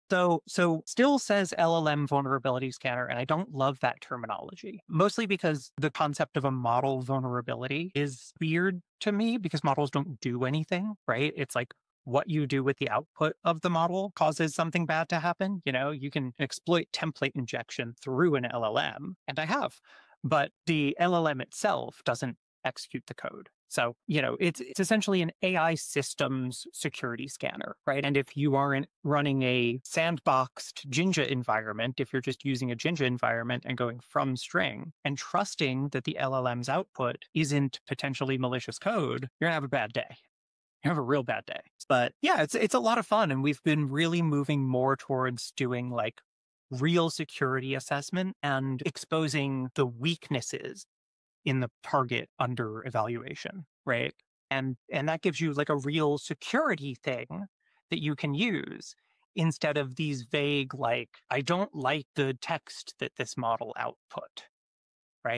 5 Crucial Facts About AI Vulnerabilities
Why calling it an 'LLM vulnerability' is misleading. It's not the model itself, but what happens with its output. Exploit template injection through an LLM—but the LLM doesn't execute the code. It's AI system security.

Unplugged's UP Phone - The performance you expect, with the privacy you deserve. Meet the alternative.